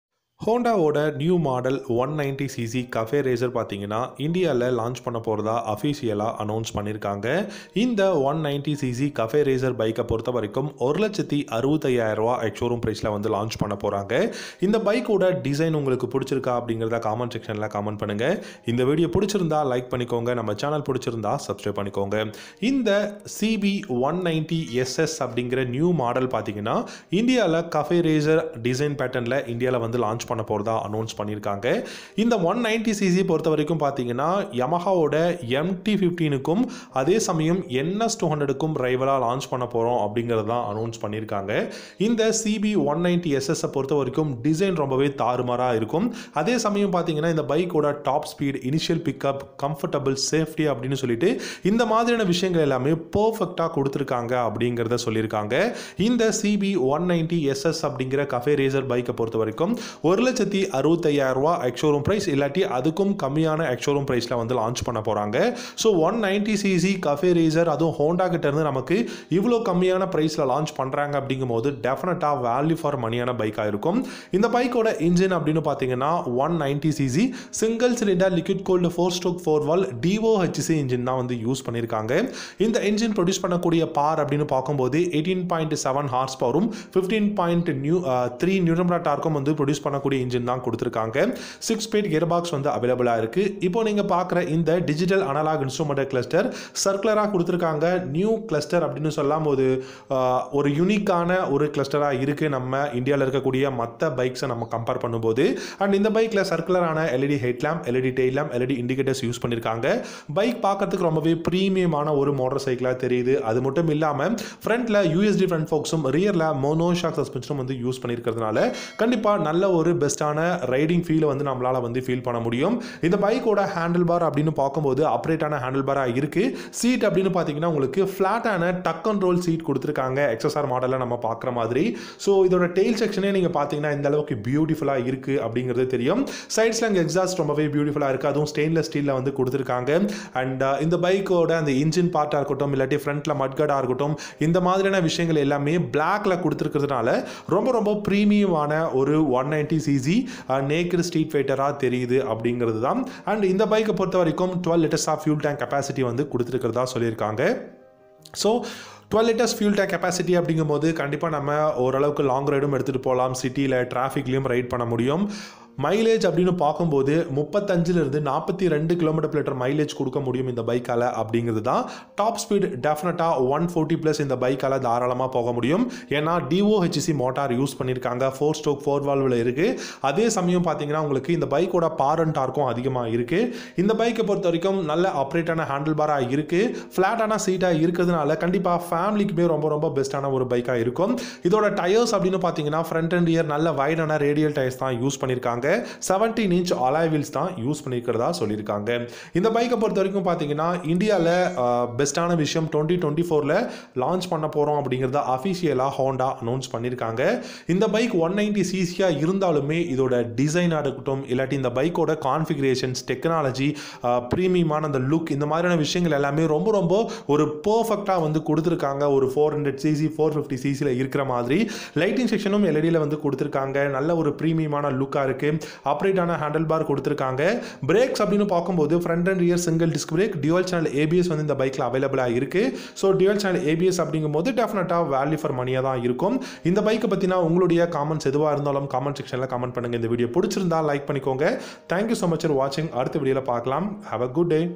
2024 Honda 190cc Bike Launched💥CB190SS Cafe Racer| Price,Spec's,Features|Epic Roads Tamil
2024 Honda 190cc Bike Launched💥CB190SS Cafe Racer| Price,Spec's,Features|Epic Roads Tamil